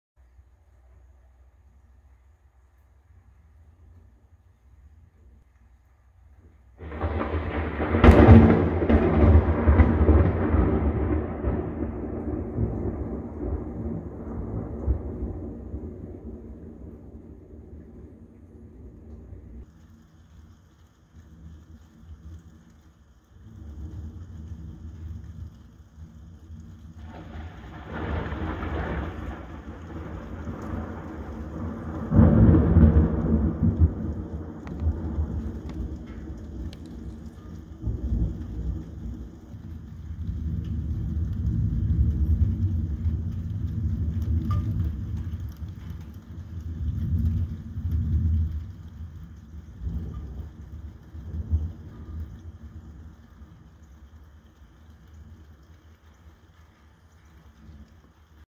RAIN LIGHTNING ROLLING THUNDER THRU WINDOW 60 seconds backyard rural Georgia best small town nature
I just missed the one that hit near my backyard & made me scream 😱

But the next 2 were pretty good too 😅

I heard all this ruckus going on over my roof - it even scared Abby (Tabby) who usually doesn't pay any attention to weather (as long as she's not out in it 😹 ).

Welp. A little more for the RainTV collection 😁

7/26/2021